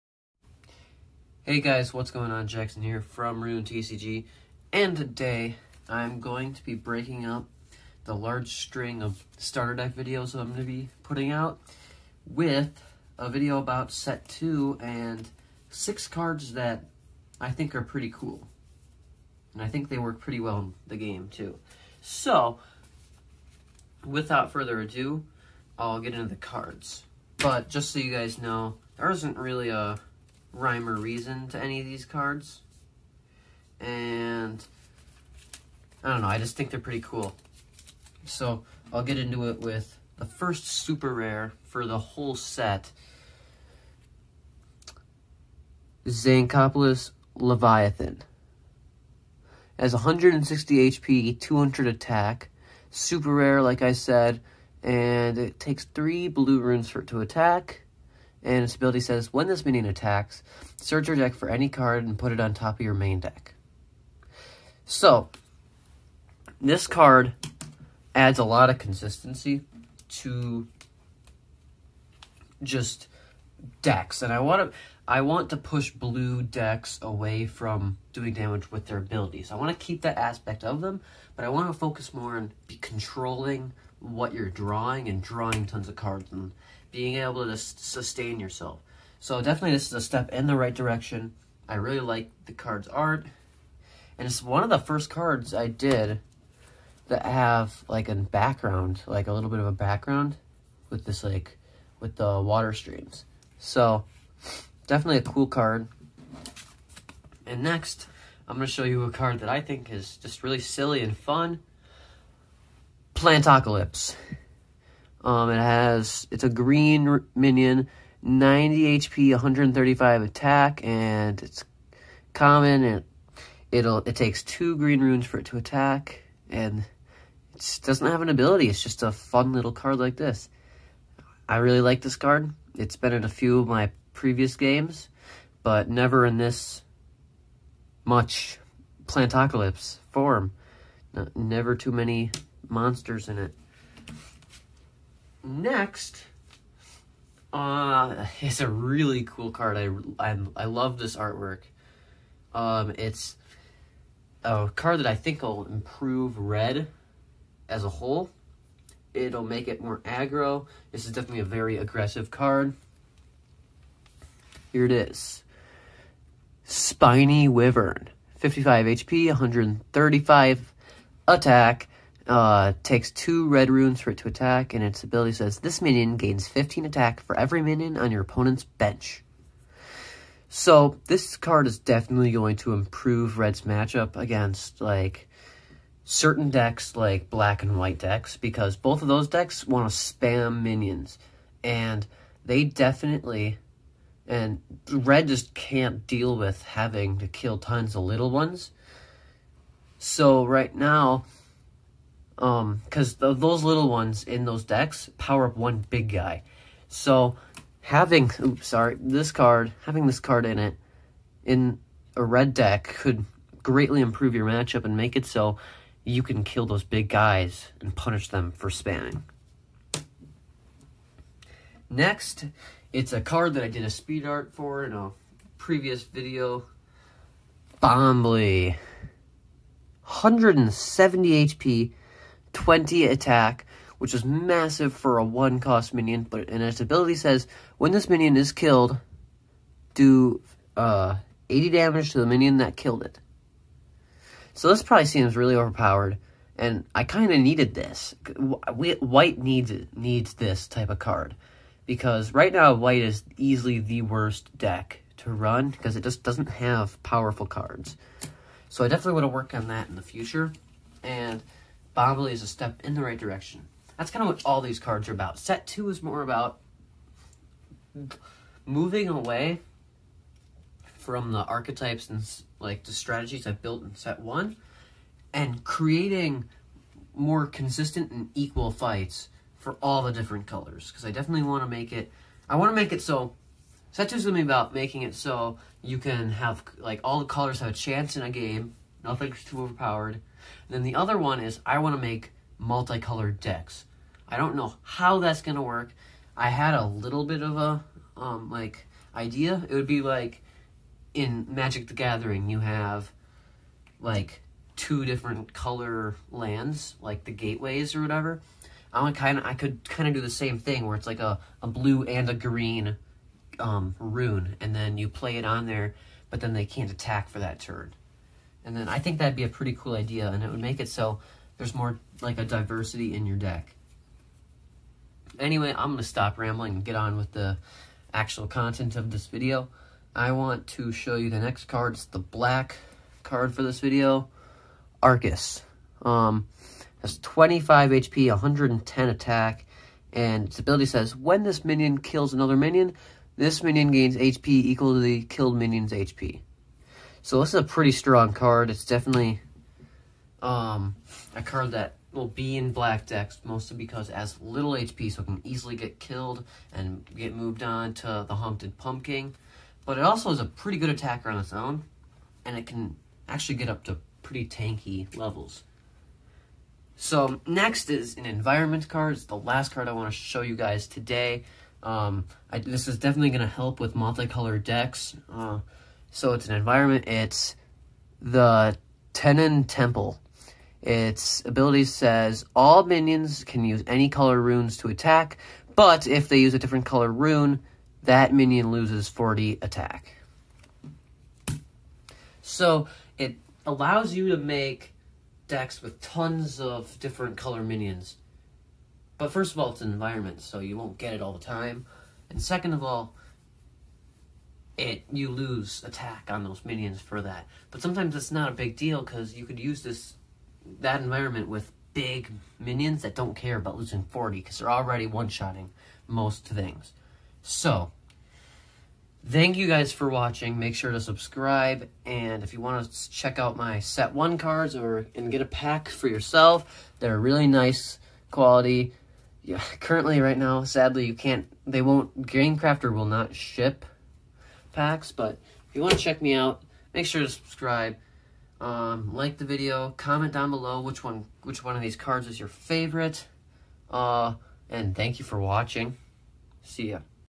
New Cards for Set 2 of my Homemade TCG!
Hey Guys! Thanks for clicking on this video, and I hope you enjoy. In this video I go over six new cards for set 2, so I have something to break up the slog of starter deck making!!